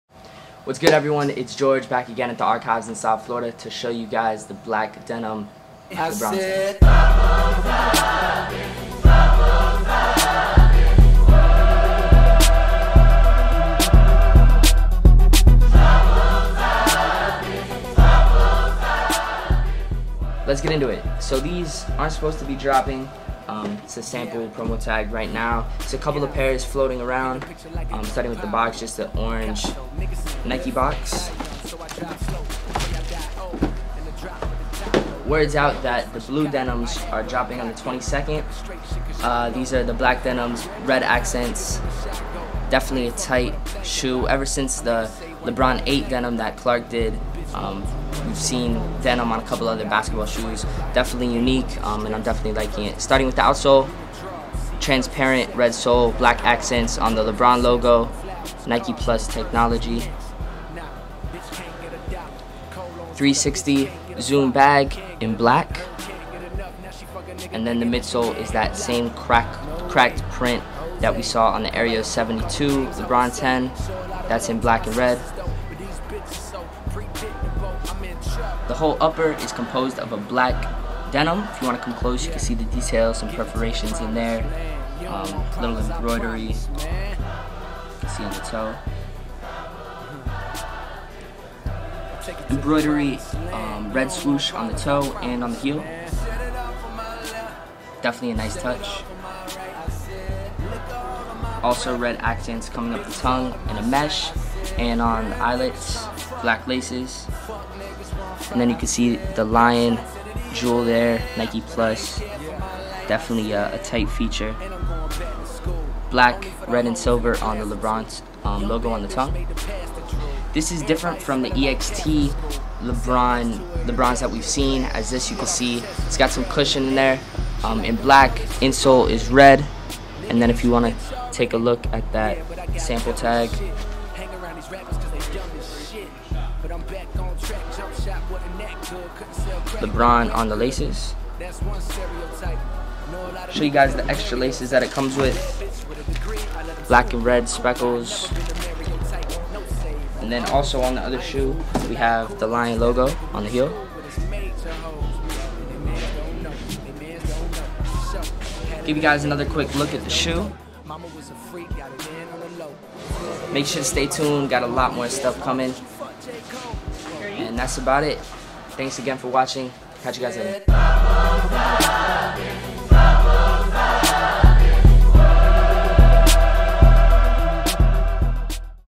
Stickie213 - Nike Black Denim Lebron 10 X Sample
Rep Code: Stickie213

The Archives
1304 East Las Olas Blvd

Video Review of Retro Nike Lebron 10 Black Denim Samples.